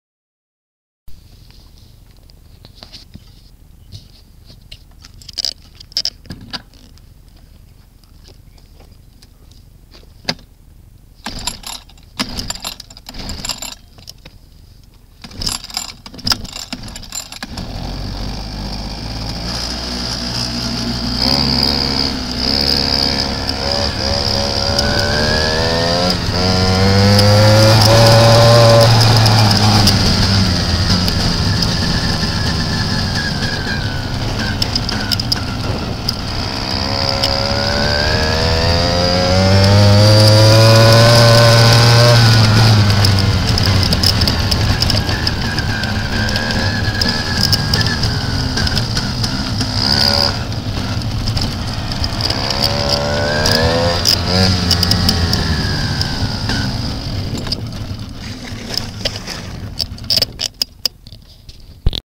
scooter cam test
testing out a action cam 3 on the track